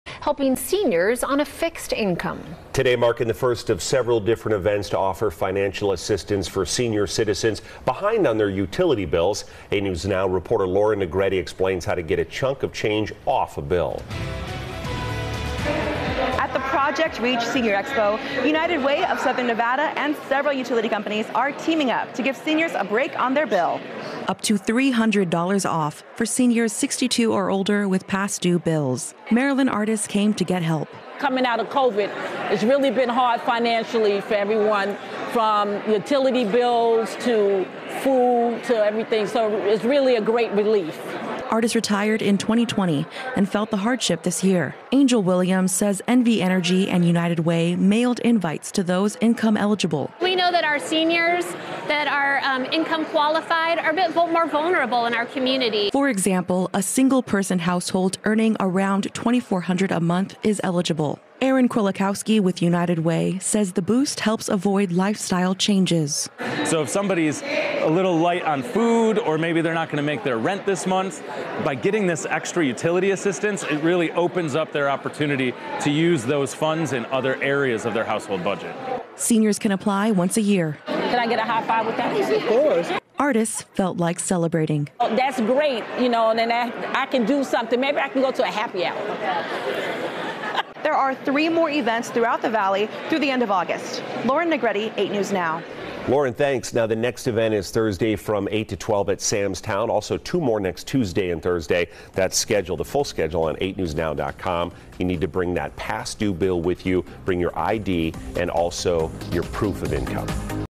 Seniors can get $300 off past due utility bills at Las Vegas expo
United Way, NV Energy, and other utility groups have teamed up to host several "Project Reach Senior Expo" events to offer seniors a break on one of their utility bills.